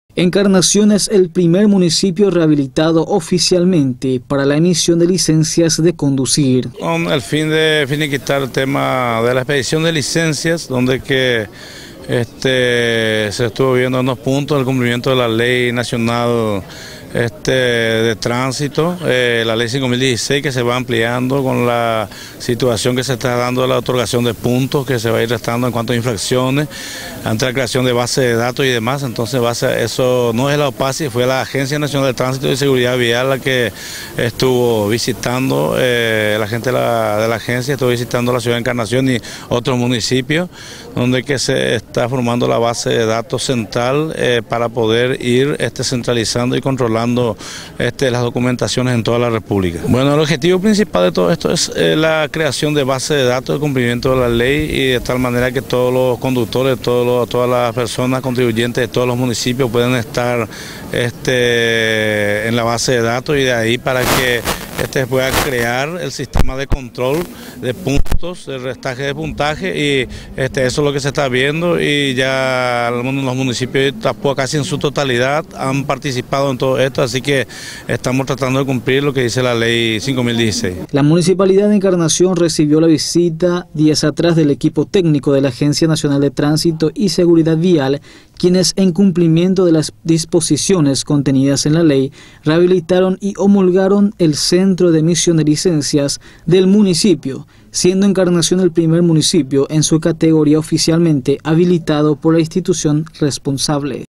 Encarnación, primer municipio rehabilitado oficialmente para la emisión de licencias de conducir
La Municipalidad de Encarnación, recibió la visita del equipo técnico de la Agencia Nacional de Tránsito y Seguridad Vial quienes, en cumplimiento de las disposiciones contenidas en la ley, rehabilitaron y homologaron el centro de emisión de licencias del municipio, siendo Encarnación el primer municipio en su categoría oficialmente habilitado por la institución responsable. Este tema también fue abordado en la reunión del intendente Luis Yd con el ministro de Obras Publicas y comunicaciones en conjunto con miembros de la OPACI en la mañana del lunes en la capital del país.